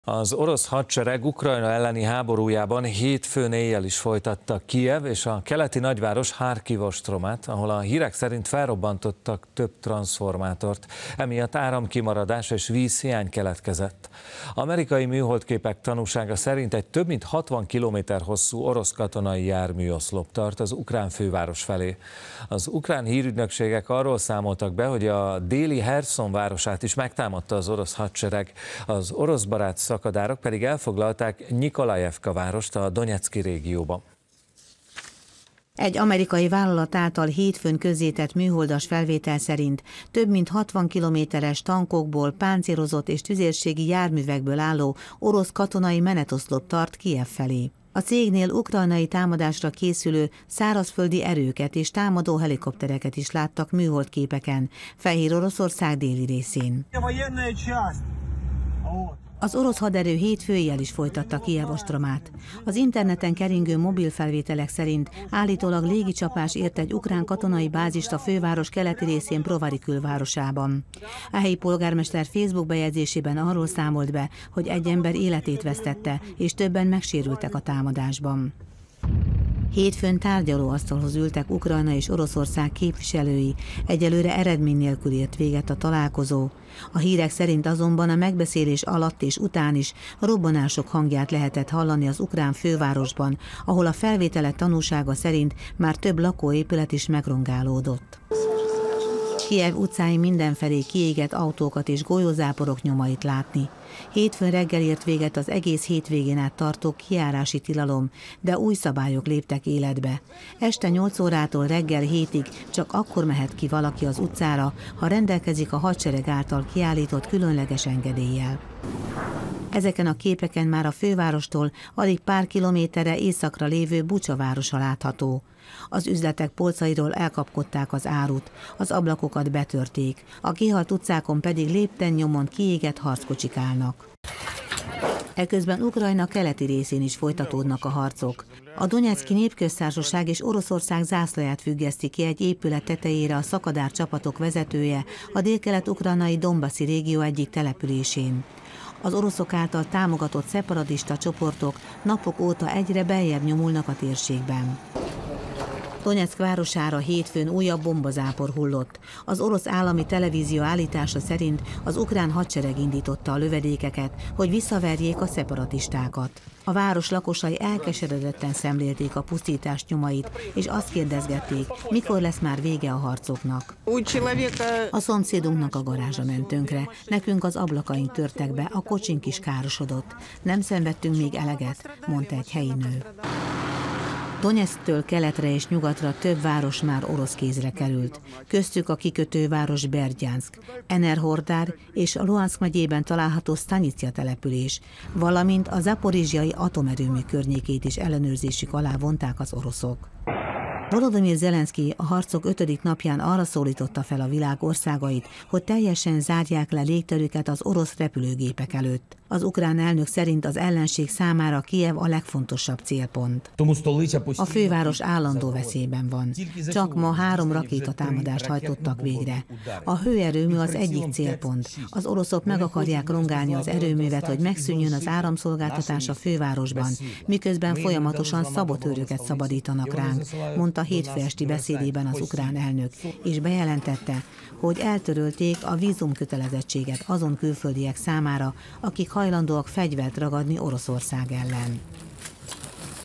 Az orosz konvoj egyre közelebb a fővároshoz
Az orosz hadsereg Ukrajna elleni háborújában hétfőn éjjel is folytatta Kijev és a keleti nagyváros, Harkiv ostromát, ahol a hírek szerint felrobbantottak több transzformátort, emiatt áramkimaradás és vízhiány keletkezett. Amerikai műholdképek tanúsága szerint egy több mint 60 kilométer hosszú orosz katonai járműoszlop tart az ukrán főváros felé. Az ukrán hírügynökségek arról számoltak be, hogy a déli Herszon városát is megtámadta az orosz hadsereg, az oroszbarát szakadárok pedig elfoglalták Nyikolajevka várost a donyecki régióban.